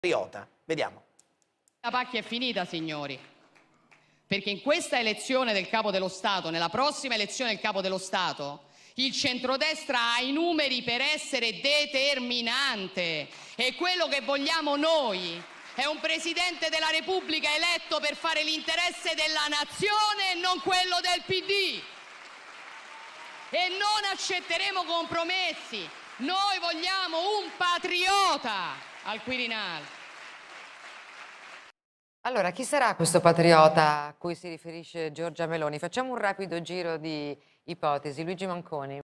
Atreju, Giorgia Meloni: "Vogliamo un patriota al Quirinale"
Giorgia Meloni dal palco di Atreju annuncia: "Vogliamo un patriota al Quirinale".